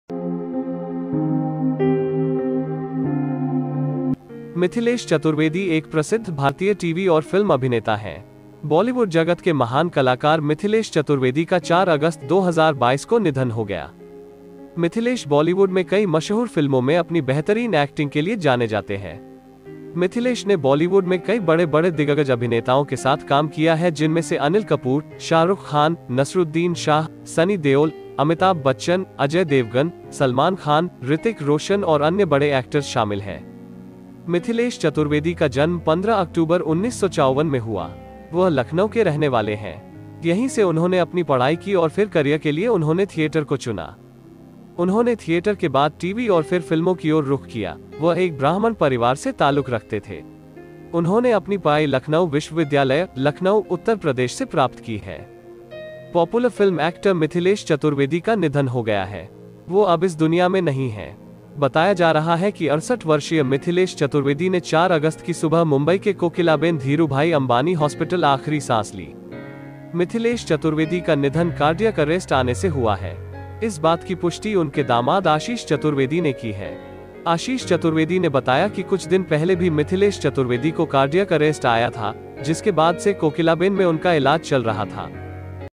Mithilesh Chaturvedi Passes away, How Did Actor Die? Chaturvedi Cause of Death
Famous Indian TV and film actor. Legendary bollywood actor MithileshChaturvedi passed away